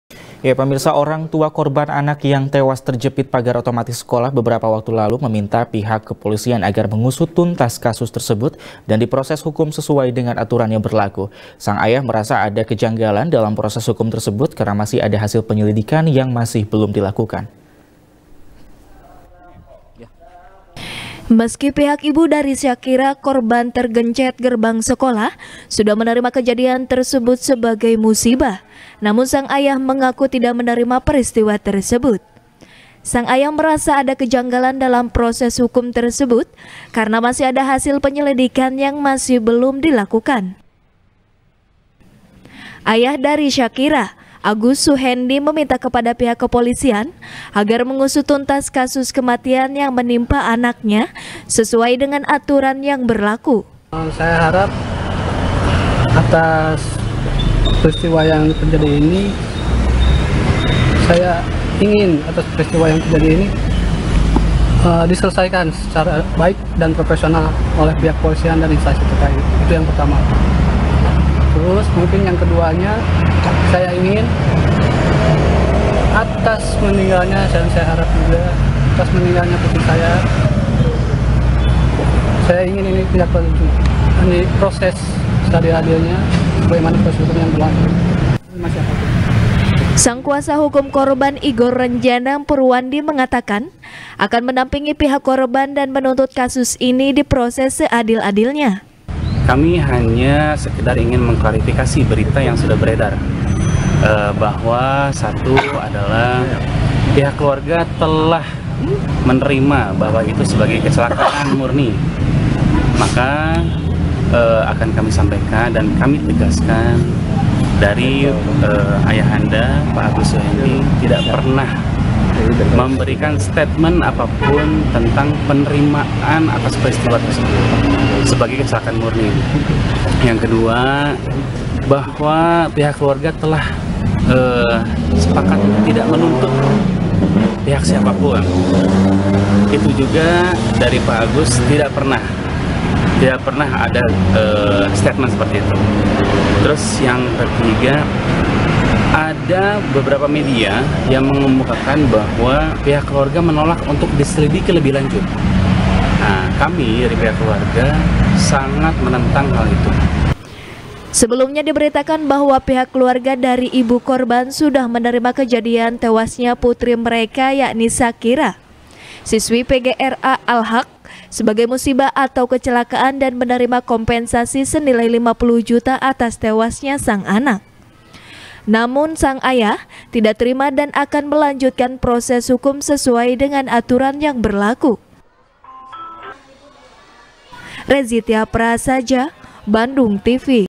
Ayah Korban Minta Proses Hukum Dilanjutkan | SBR | BANDUNGTV
SBR | BANDUNGTV | IRFAN RAMANDA | Pemirsa,Orang Tua Korban Anak Yang Tewas Terjepit Pagar Otomatis Sekolah,Beberapa Waktu Lalu,Meminta Pihak Kepolisian Agar Mengusut Tuntas,Kasus Tersebut,Dan Diproses Hukum Sesuai Dengan Aturan Yang Berlaku.Sang Ayah,Merasa Ada Kejanggalan Dalam Proses Hukum Tersebut,Karena Masih Ada Hasil Penyelidikan Yang Masih Belum Di Lakukan